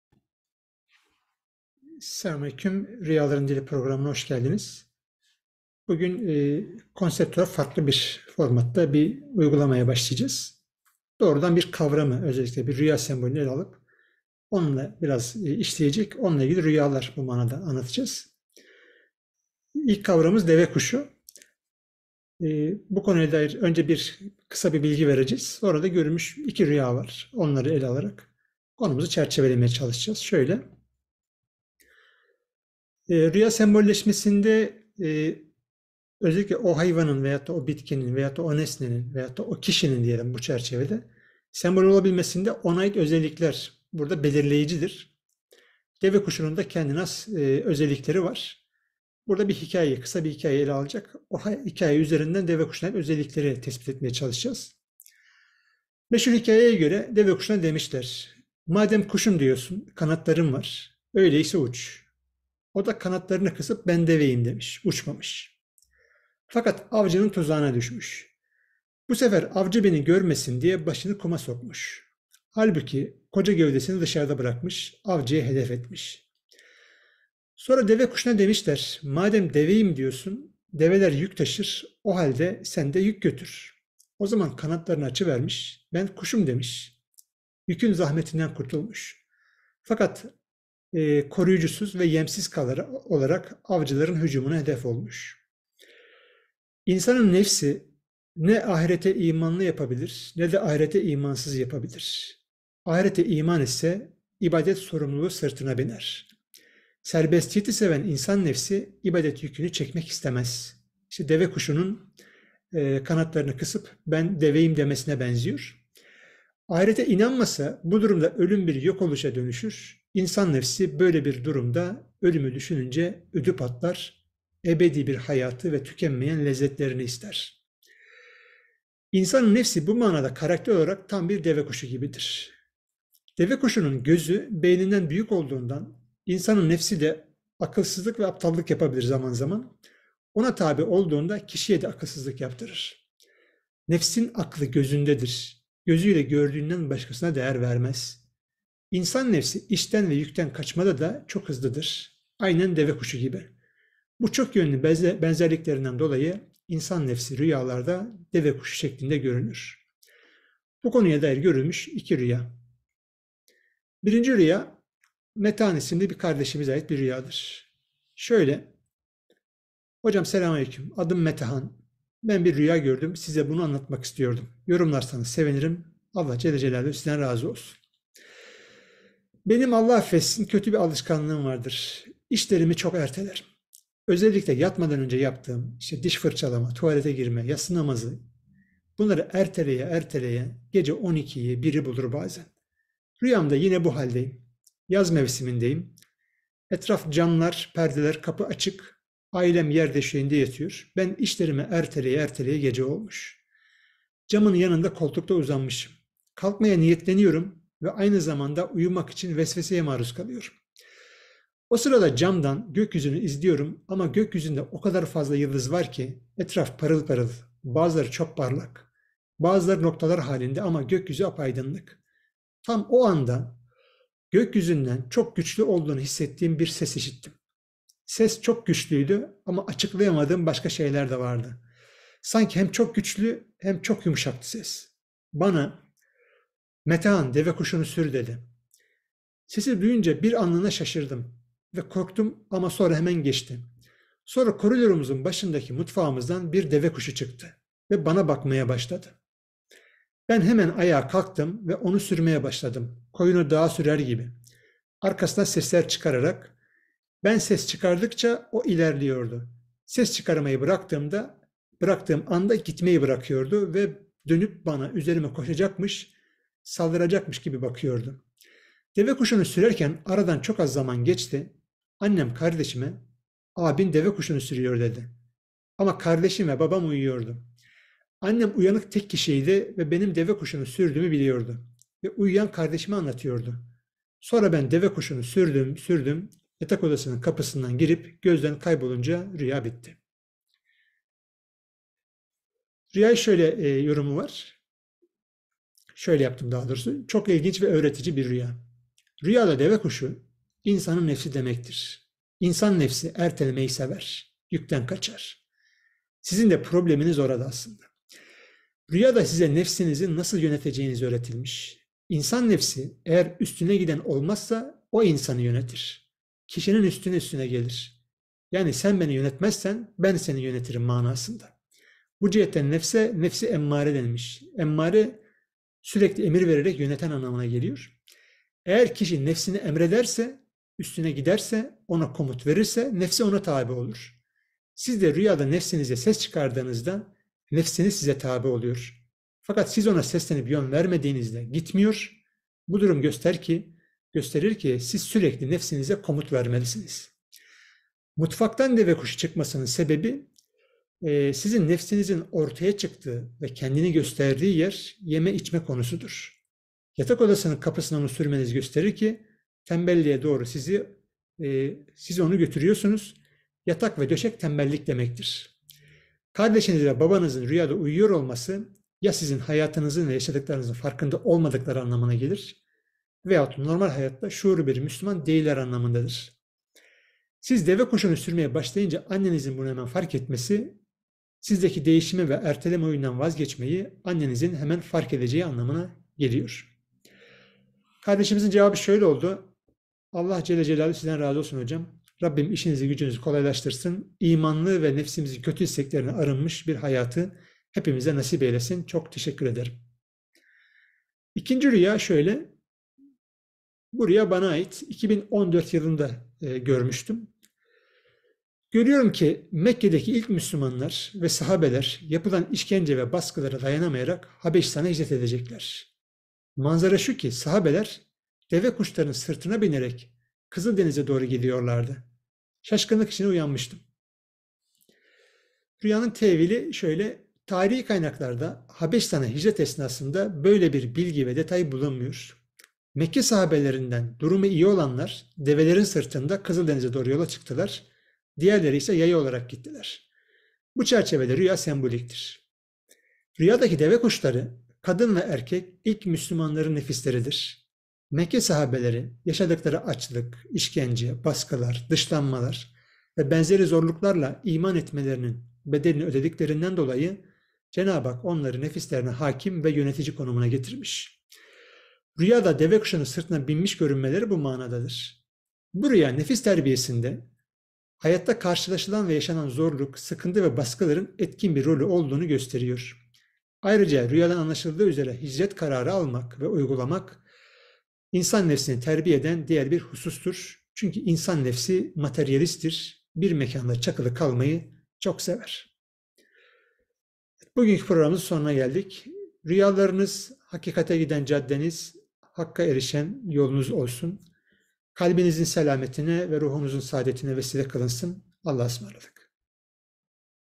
Rüyada Devekuşu Görmek (Mutfak, Yatak, Döşek, Sahabeler, Hicret)
Rüyalarda görülen hayvanlar çeşitli konularda insan ile alaklı hakikatlerin sembolleridirler. Devekuşu da bu rüya sembollerinden birisidir. Bu videoa hem bu sembolleşme mantığını ve gerekçelerini ele alacak, hem örnek rüyalar üzerinde meseleyi netleştirmeye çalışacağız. Gayret bizden muvaffakıyet Allah'tandır.